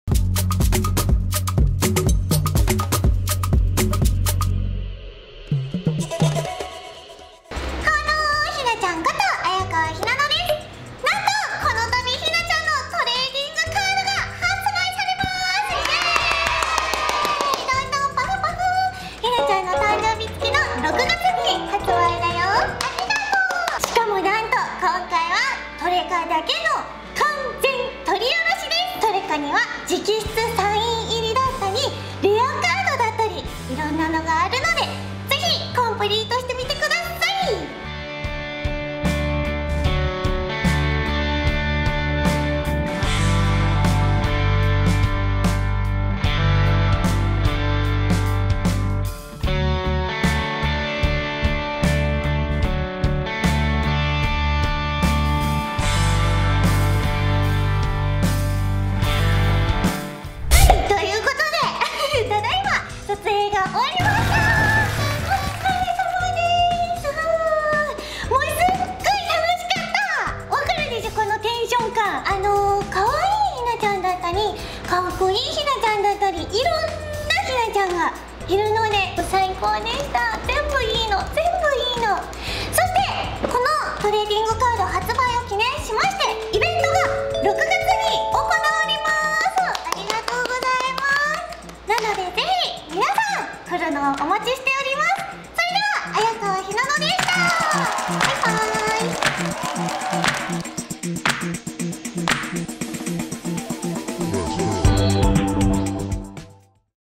【ハレンチボディ💖】グラビア撮影メイキング！彩川ひなのファースト・トレカ2021年6月20日発売！
魅惑のハレンチボディ”彩川ひなの”がトレカにやってきた♪

トレカも“うぇるかむ”♪・・・ひなりんセカイ全開で萌え萌えキュン♪♪
「可愛いからカッコいいまで全部いいの全部いいの！」と本人も大絶賛！！
そんなひなりんトレカには「直筆サインカード」や「生キスカード」のレアカードは当たり前！

今回はなんと！超レアな「ストッキングパーツカード」も入れちゃいます！
そしてそして撮影時に使用したビキニを大量封入！大好評「ピンスポビキニカード」も！！

発売日は2021年6月20日ー記念すべきひなりんバースデイ・・・
ハッピーバースデイ&ファースト・トレカ！！